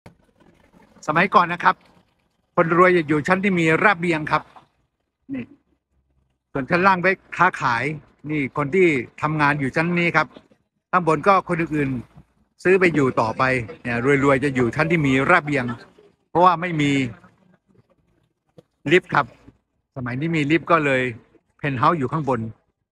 ทำไมตึกฝรั่ง 200 ปีขึ้นไปมักมีระเบียงที่ชั้น 3
ท่านสังเกตไหมตึกเก่าๆ แต่แข็งแรงที่สร้างมา 200 ปีขึ้นไปในประเทศตะวันตก (เช่นที่ยกมาบรรยายตามคลิป) จึงมักมีระเบียงที่ชั้น 3 ดร.โสภณ พรโชคชัย ประธาน ศูนย์ข้อมูลวิจัยและประเมินค่าอสังหาริมทรัพย์ไทย บจก. เอเจนซี่ ฟอร์ เรียลเอสเตท แอฟแฟร์ส มาแจกแจงให้ฟัง